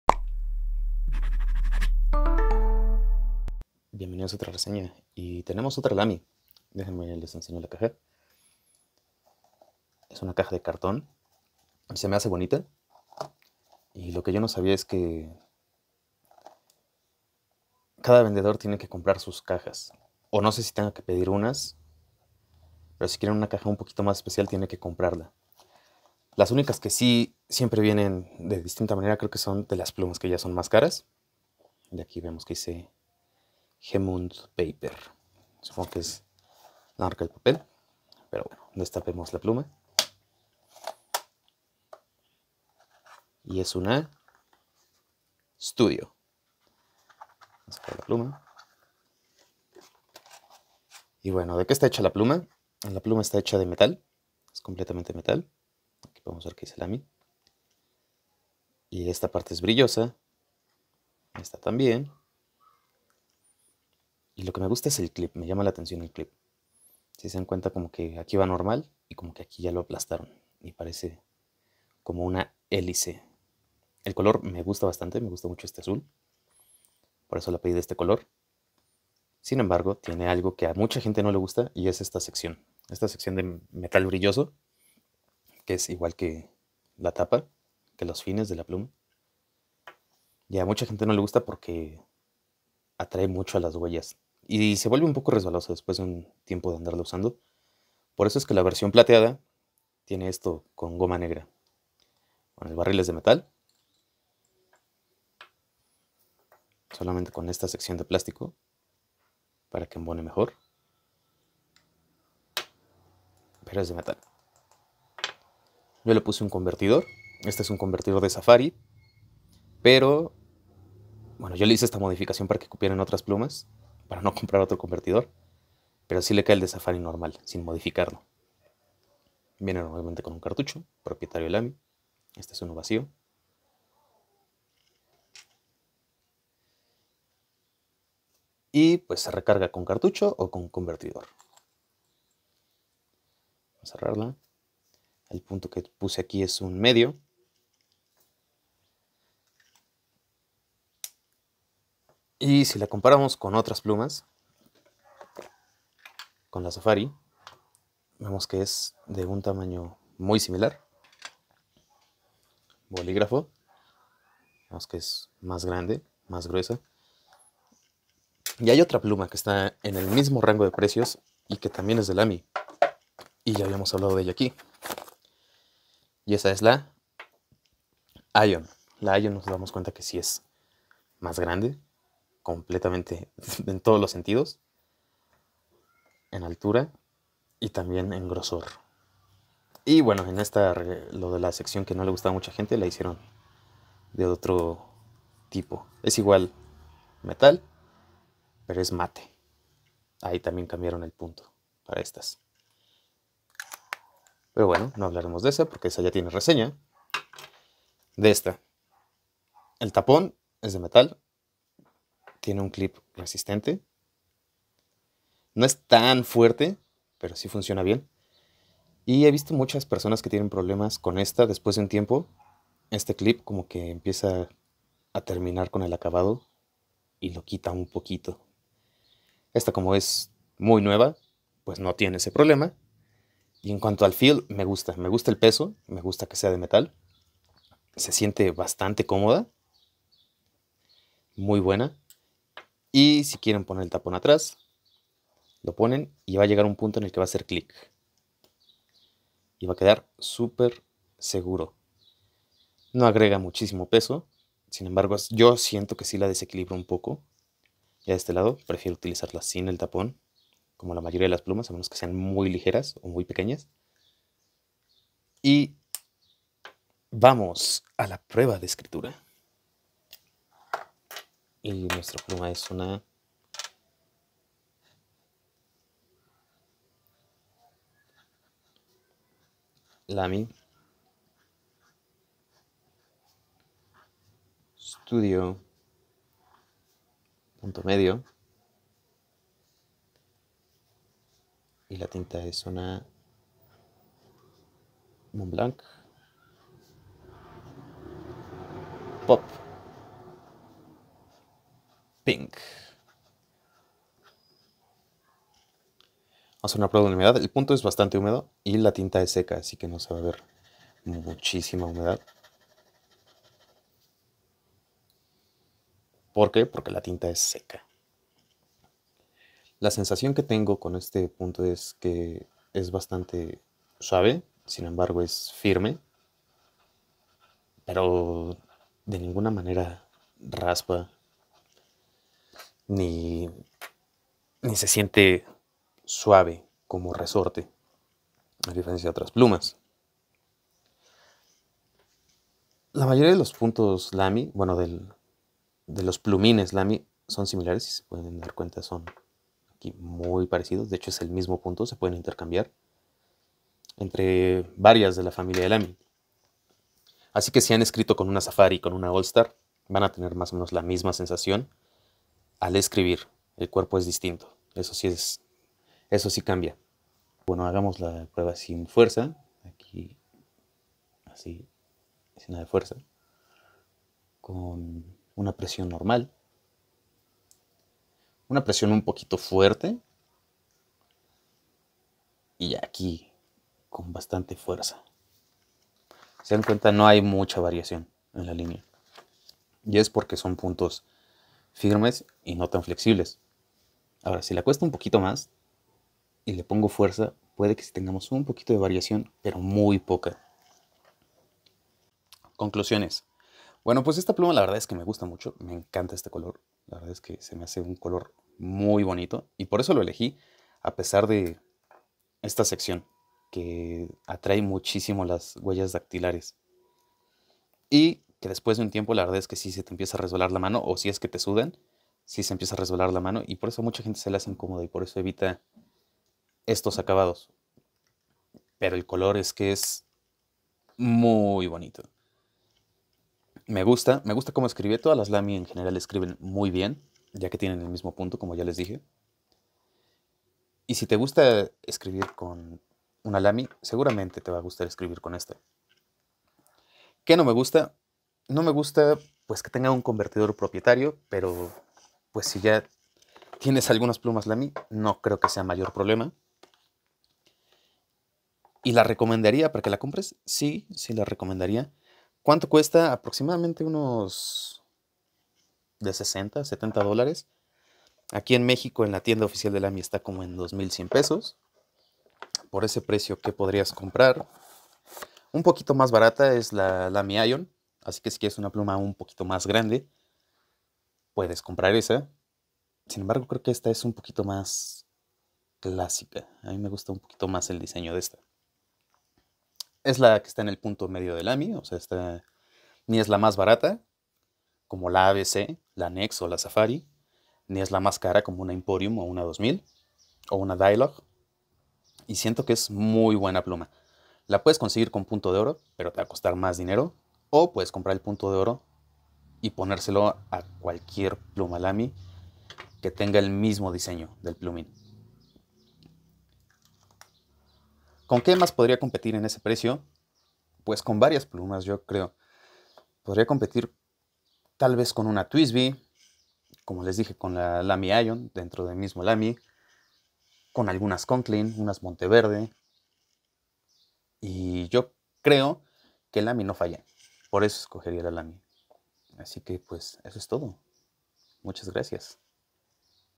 Lamy Studio Pluma Fuente/Estilográfica reseña en español
La gama media de Lamy no decepciona.